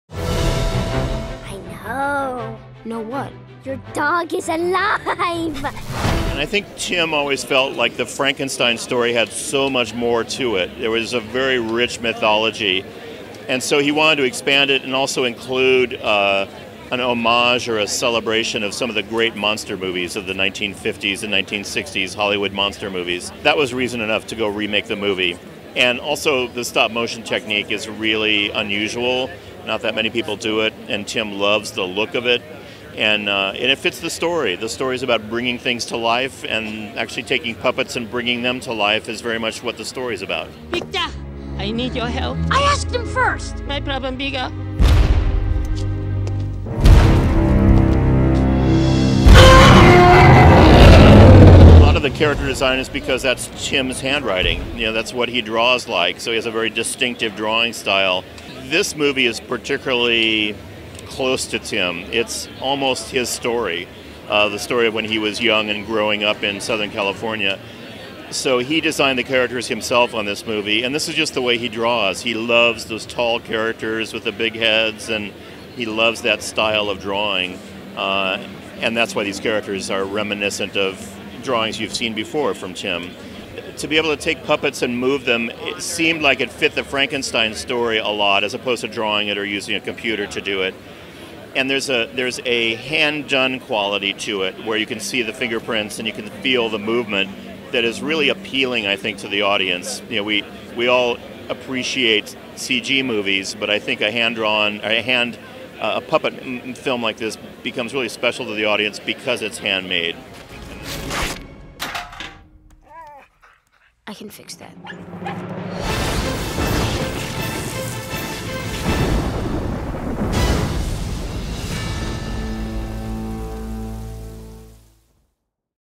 Продюсер "Франкенвини" Дон Хэн о кукольной анимации
ЭКСКЛЮЗИВ: Сан-Диего превратился в царство «Франкенвини». Возле сердца Комик-Кона, выставочного центра, было разбито настоящее анимационное кладбище домашних животных, в Hall H ленту представлял Тим Бертон, а на выставке кукол и эскизов из мультфильма мы поговорили с его продюсером Доном Хэном.